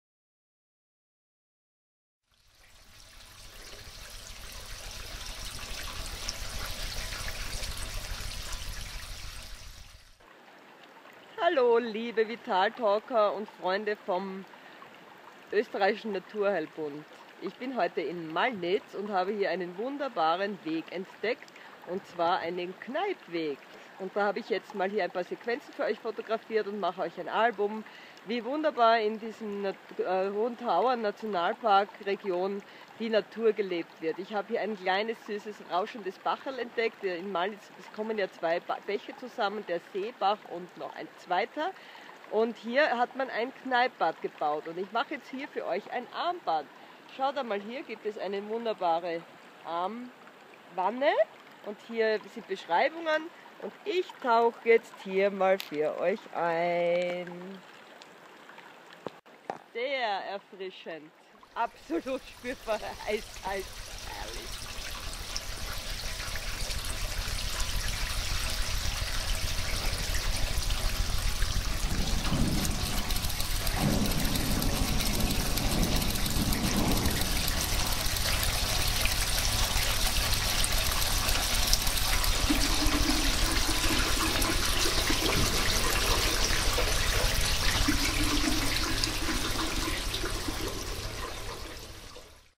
Bezauberndes Kneippen in Mallnitz/Kärnten - Nationalpark Hohe Tauern
WASSER IST LEBEN! KNEIPPEN ist eine der besten Grundlagen zur Vermeidung von Gefäßerkrankungen, Heilung von bestehenden Störungen und macht einfach GUTE LAUNE! In der Dusche oder wie hier im Freien: In Mallnitz gibt es diesen besonders schönen Kneipp-Pfad!! Das Wasser direkt vom Tauern ist ein Juwel der Natur und für mich oberstes Premium-Gut auf der Welt. Einen kurzen Moment habe ich für Euch eingefangen - fahrt hin und kneippt dort selbst!
Weitere Infos zu diesem Film und Mallnitz im Fotoalbum auf FacebooK: Vitaltalk - Patricia Fromme Bloggerin - Album vom 4.9.2016